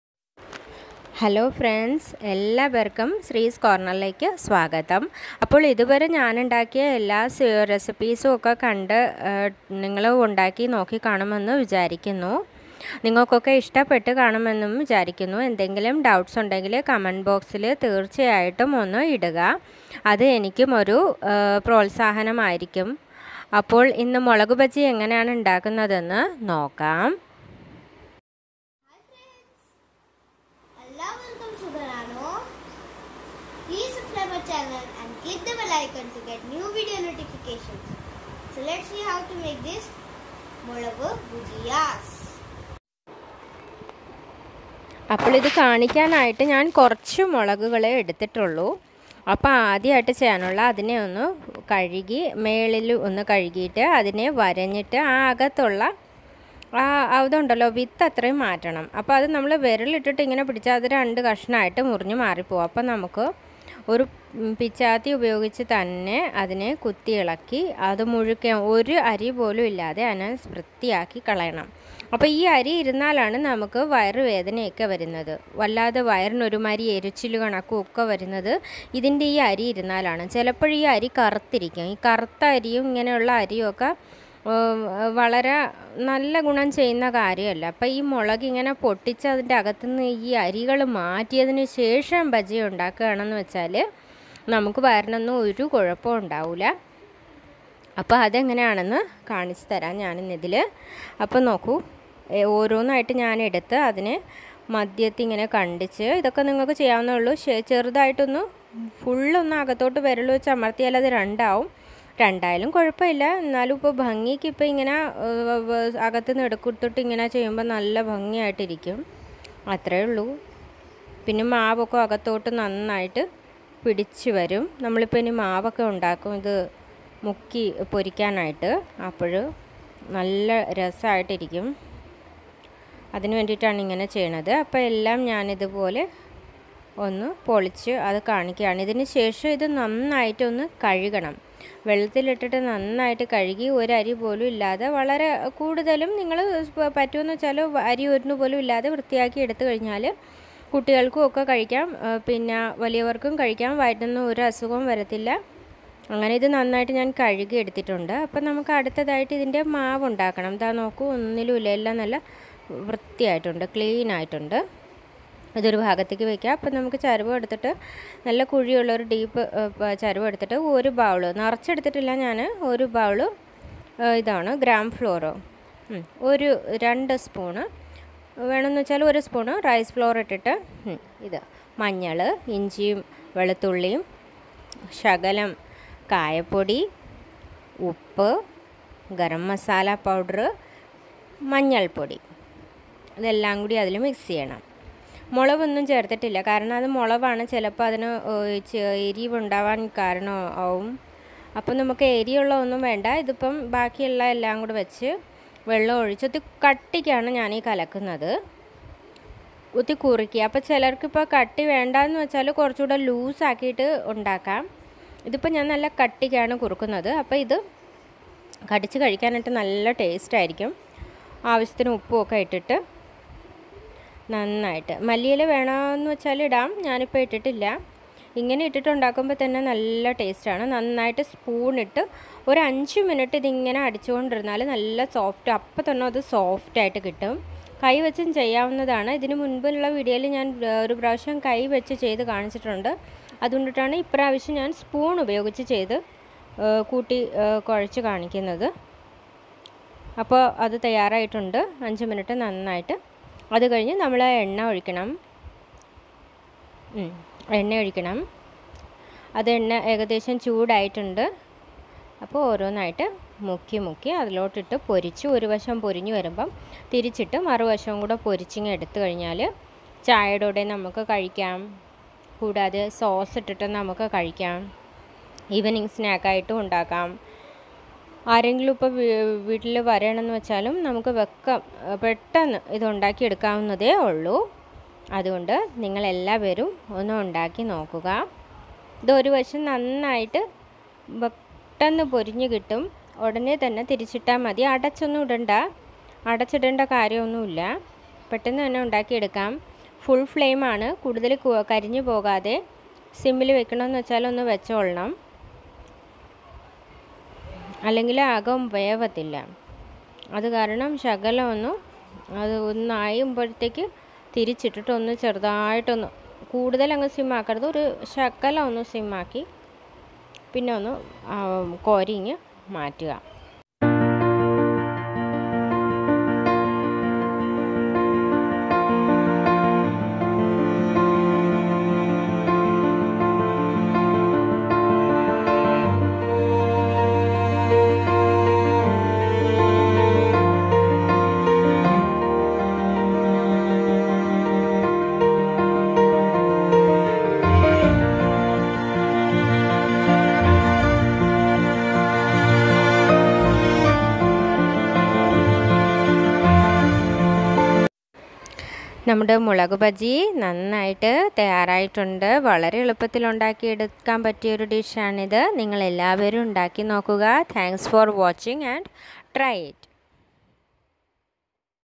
Mulaku bhajji/ Mulaku porichatu/ Chilli bhajiyas
INGREDIENTS

Chillies
Gram flour
Rice flour
Asafoetida
Turmeric powder
Garam masala

Method of making

Clean the chillies and remove the seeds inside it so that it will make
Stomach pain. After cut and clean it
 Take a deep vessel add 1 bowl gram flour, 1 spoon rice flour and add ingredients of turmeric powder, ginger garlic paste, Asafetida , salt, Garam_ Masala into it and mix it well with water using spoon or hands.
Dip the chillies into the paste and
 Deep fry it.
Enjoy it with sauce. Yummy and delicious chillies are ready. 👍 👍
Try it..